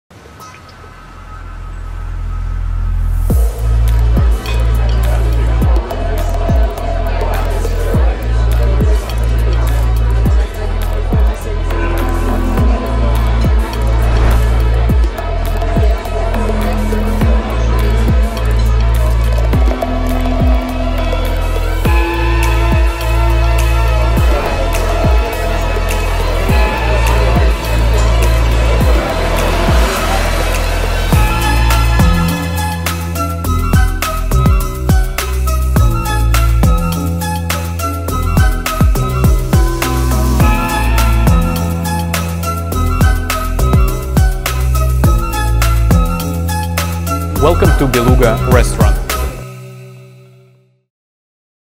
Welcome to Beluga Restaurant
«Beluga» is the first restaurant of Russian delicacies and caviar brasserie. Alexander Rappoport, the famous restaurateur and innovator, getting a new angle on the theme of the Russian cuisine. It’s located on the second floor of the Hotel National Moscow and has perhaps the most luxurious caviar bar in the world. There are dozens of all different types of caviar, served both as the esthetic tasting sets, and as the nostalgic sandwiches with butter. «Beluga» restaurant is perfect to hold a business meeting, a romantic date, to meet friends or have a dinner with foreign guests. You can get away, receive positive feelings, impress, surprise and at the same time — not go broke, because prices at «Beluga» are quite reasonable in spite of all the luxury of the concept, the exclusivity of the panorama, the elegance of products and the beauty of the decoration.

Beluga
Russian delicacies and caviar brasserie
15/1 Mokhovaya str. bld. 1, Moscow, 125009 (Hotel National Moscow, 2d floor)